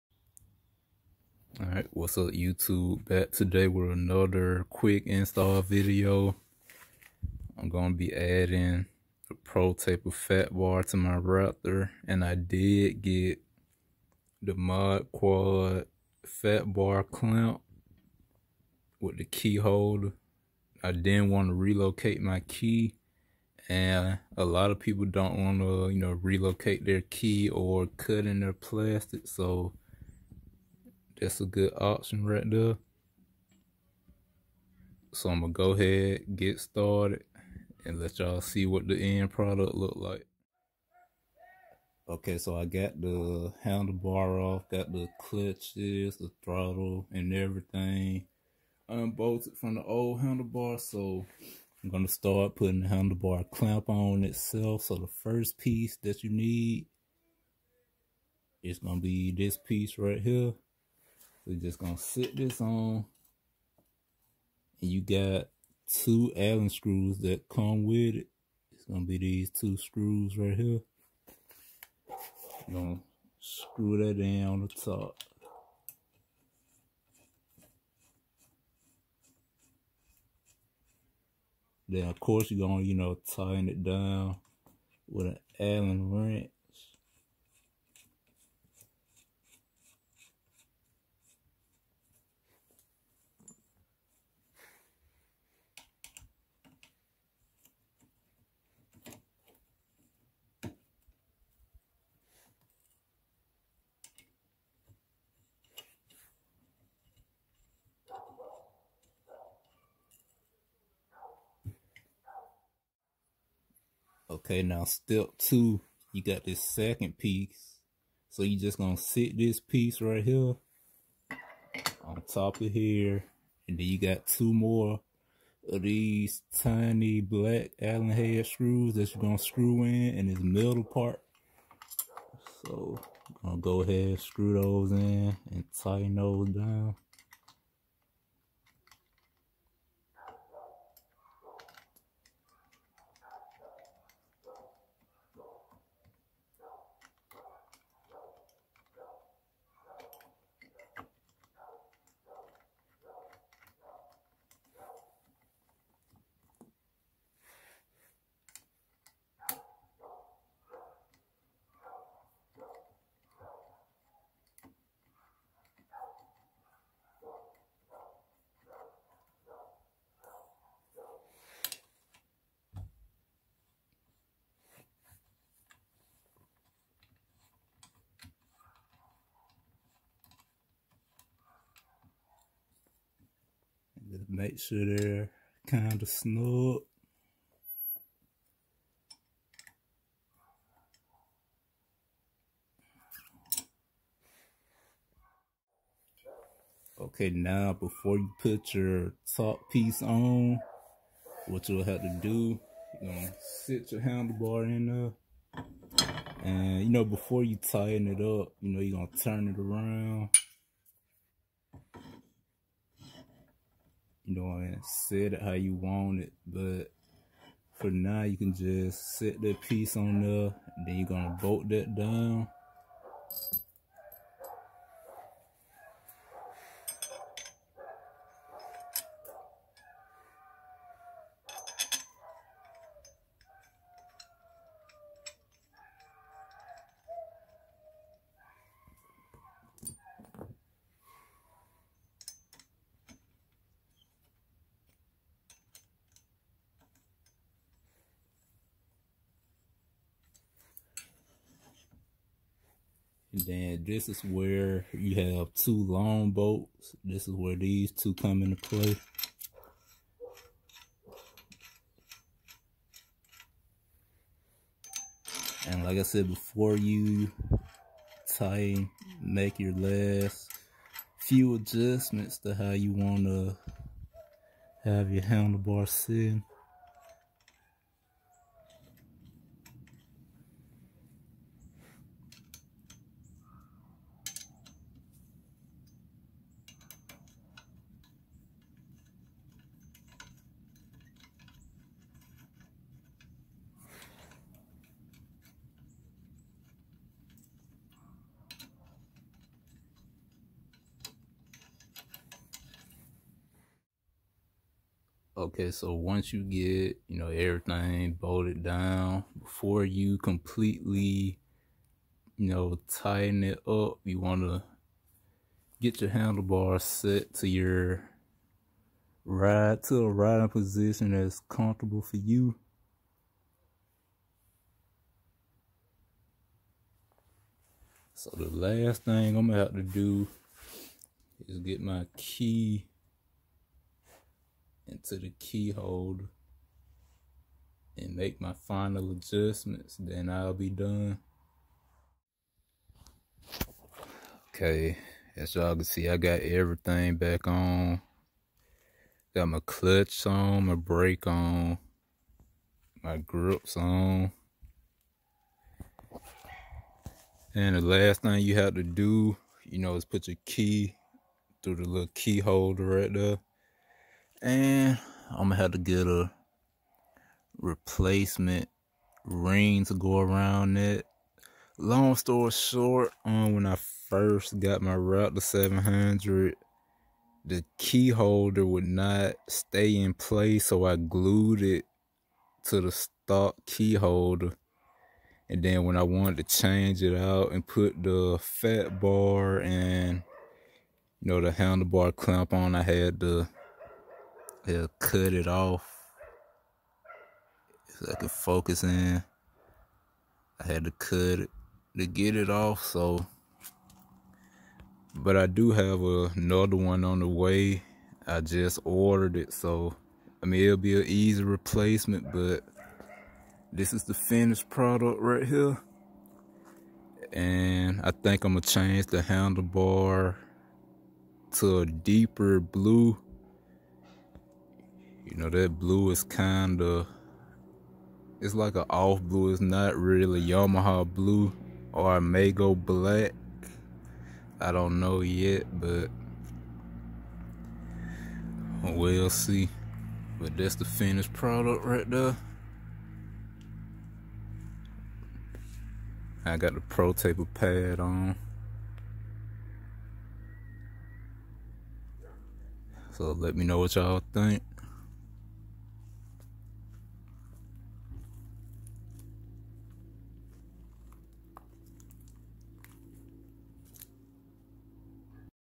Protaper Fatbar/Modquad Fatbar Clamp Install Raptor 700
Installing Protaper Fatbar With Modquad Fatbar Clamp